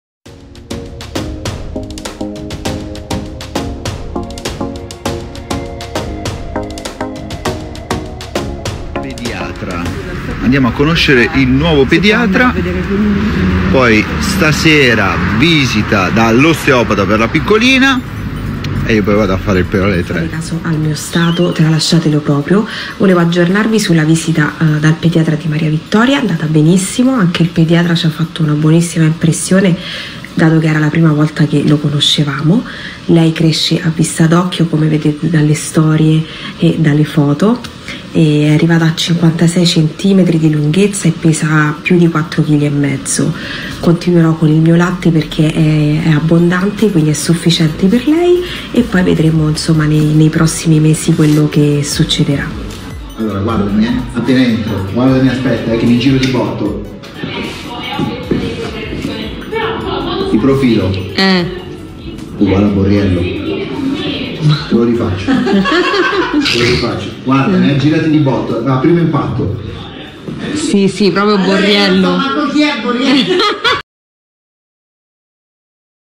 Uominiedonne Lorenzo Riccardi e Claudia Dionigi portano baby Cobrina dal pediatra
E adeso vi mostriamo questo video che ci mostra Lorenzo Riccardi e Claudia Dionigi che hanno portato Lady Cobrina a fare vedere da un pediatra e le cose stanno andando benissimo per la felicità dei genitori.